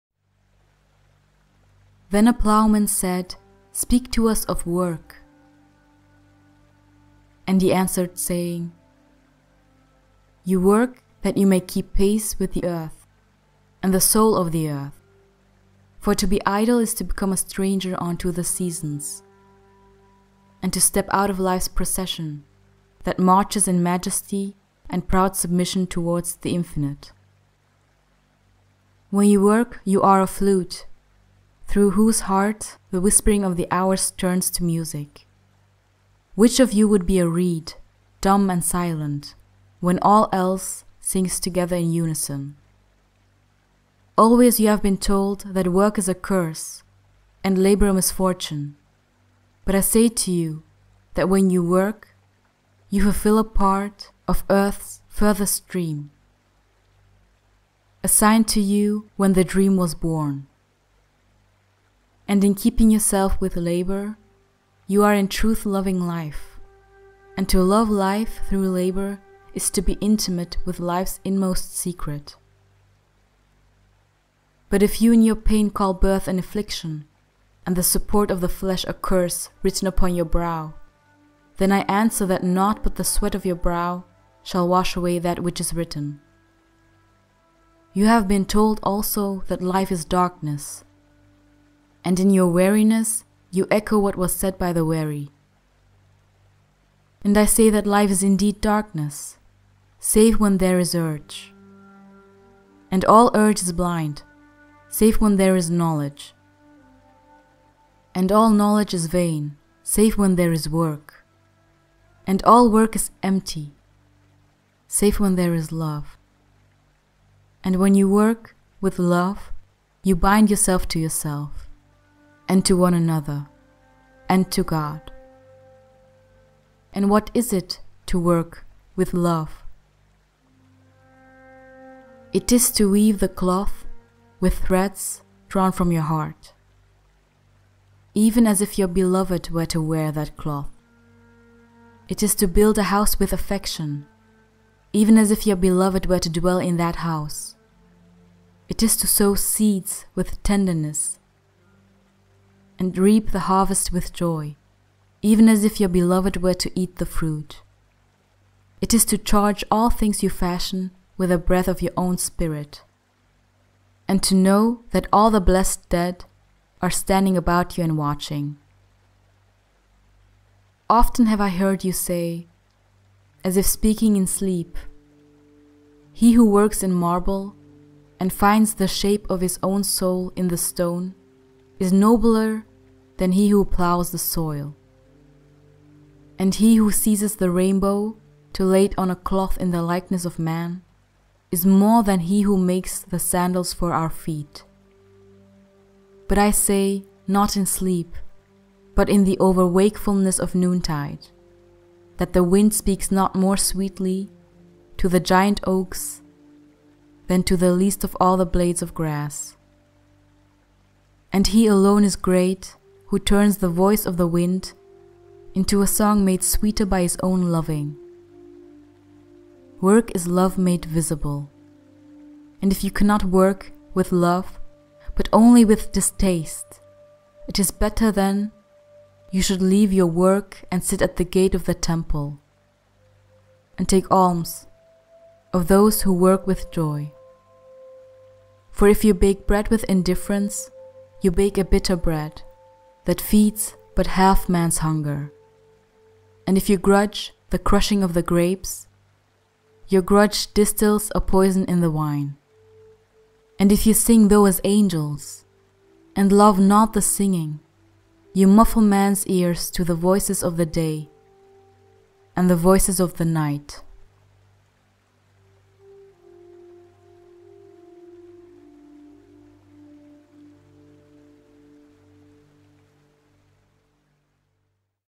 The Prophet by Khalil Gibran (pt.7) - Work
"And if you sing though as angels, and love not the singing,
you muffle man's ears to the voices of the day and the voices of the night."

Seventh chapter of the book on work.